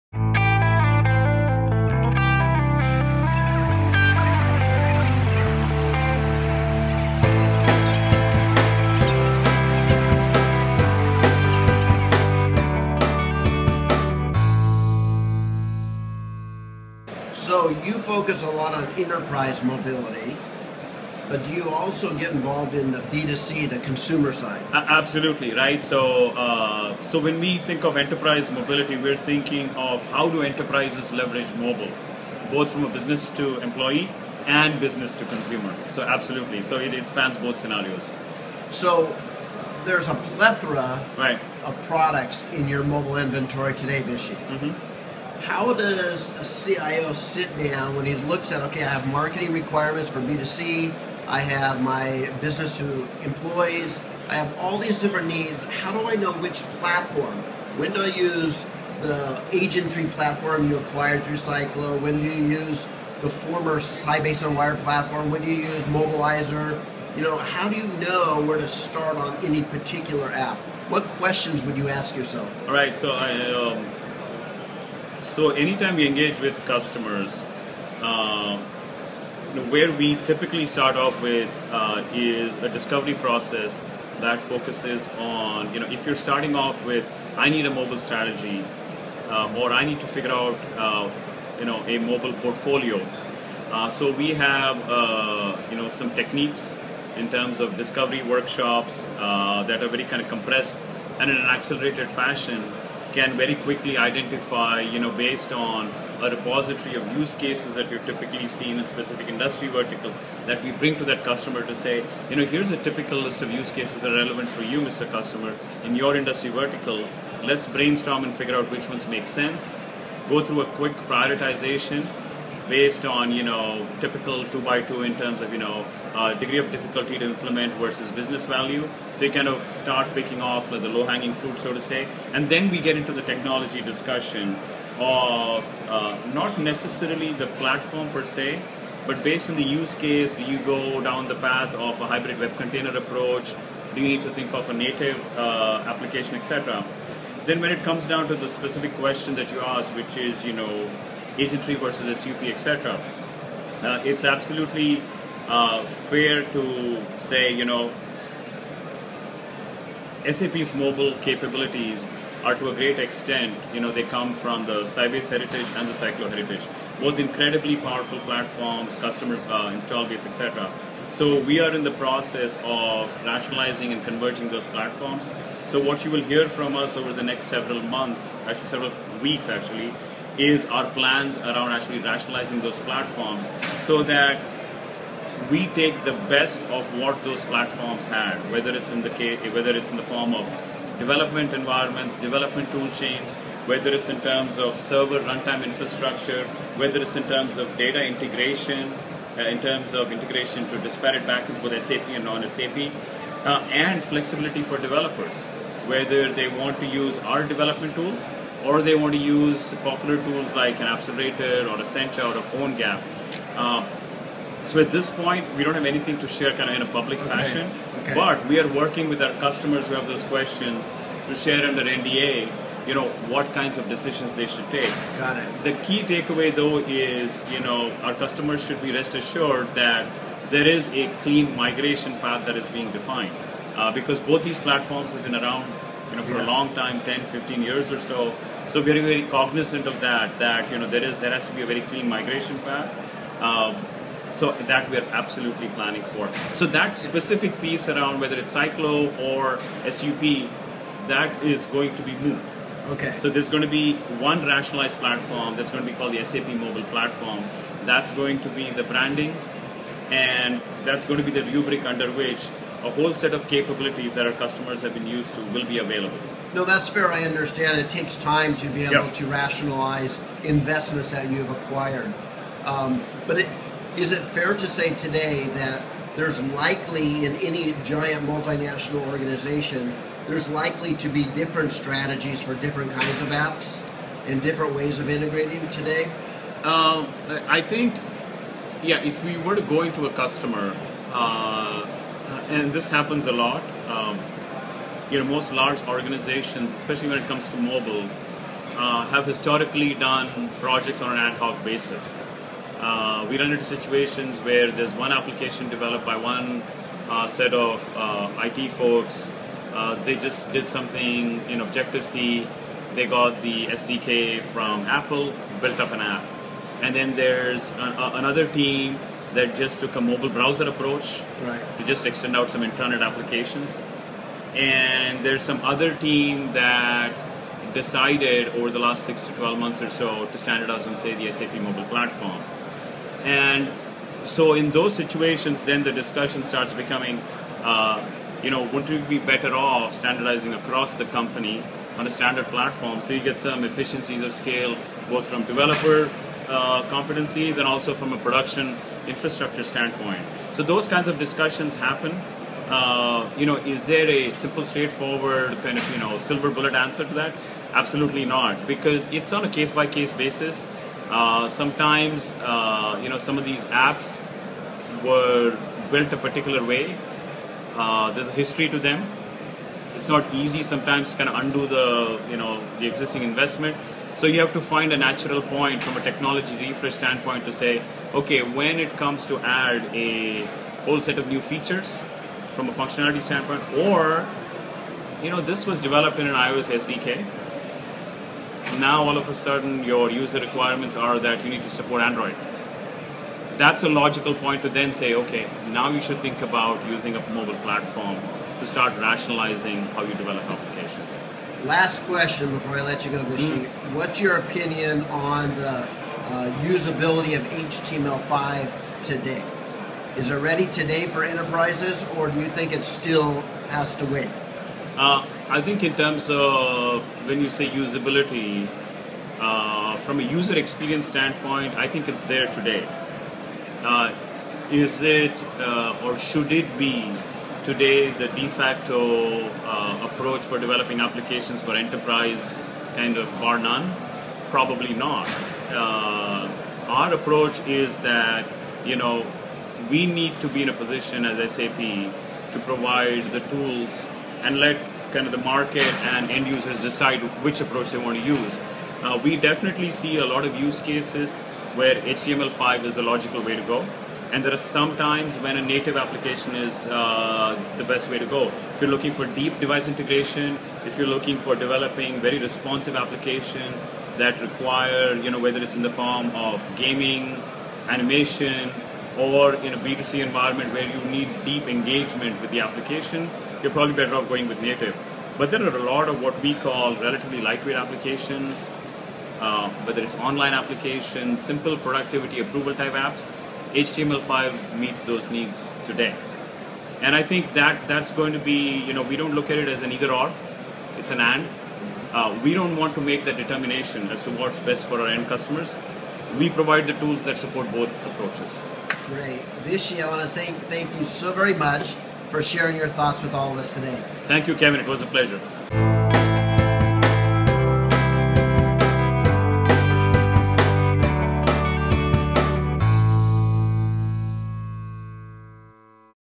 Mobile World Congress 2013 SAP's Vishy Gopalakrishnan Pt 3
Cognizant SMAC Analyst Kevin Benedict interviews SAP's VP of Global Center of Excellence - Mobile Solutions on strategies for determining best mobile platform and development strategies.